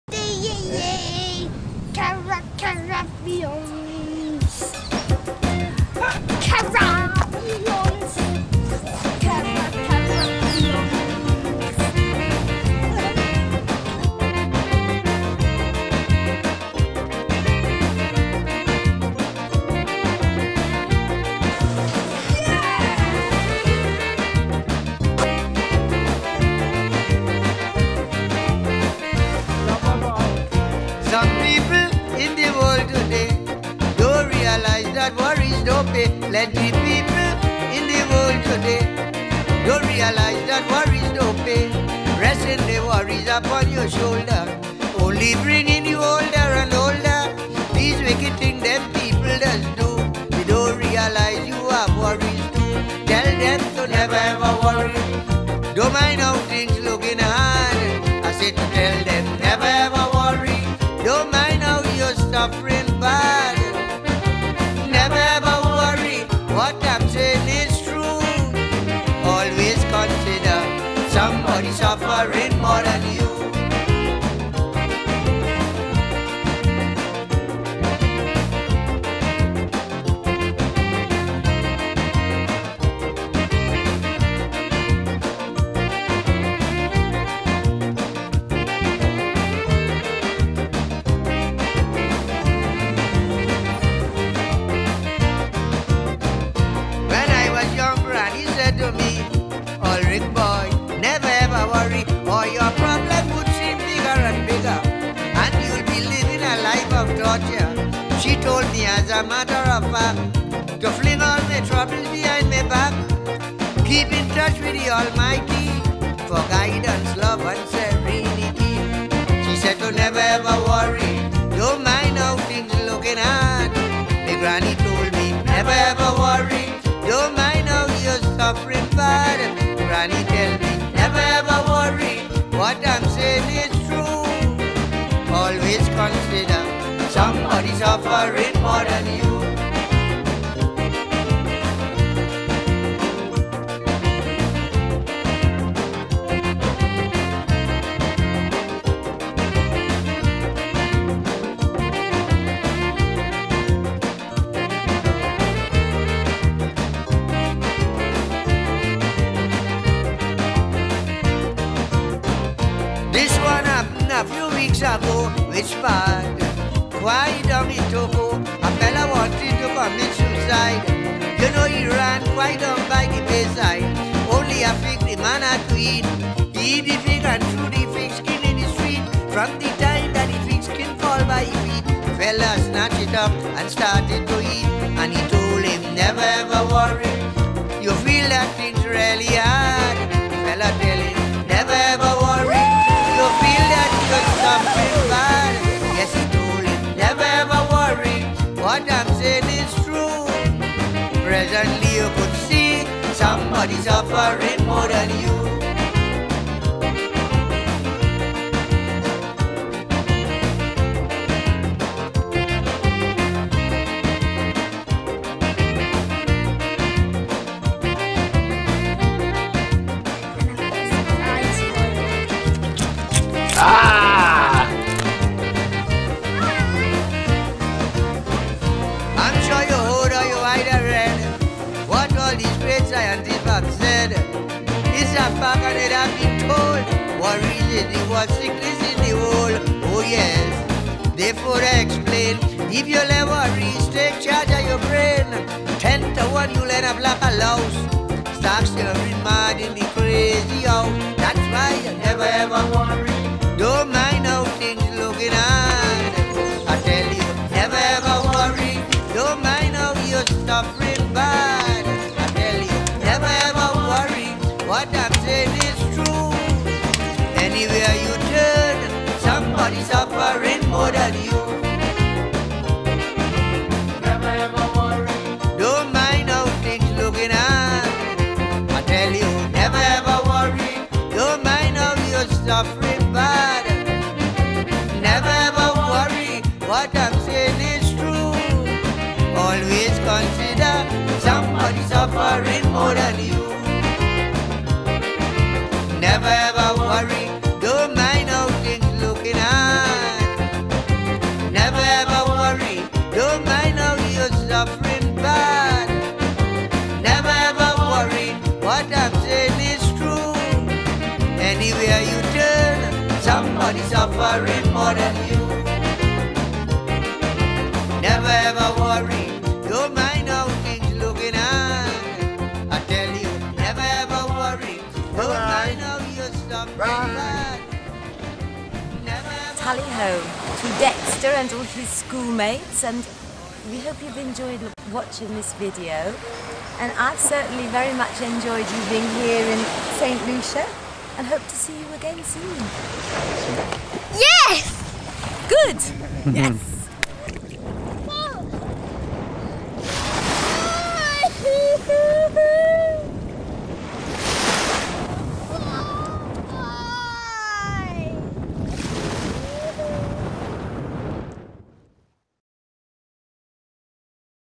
Dexter in St Lucia
In 2002 I took my son Dexter Pletts to St Lucia, where we stayed with a lovely local family and went to my half sister's wedding.  This is the end of a half hour family video I shot.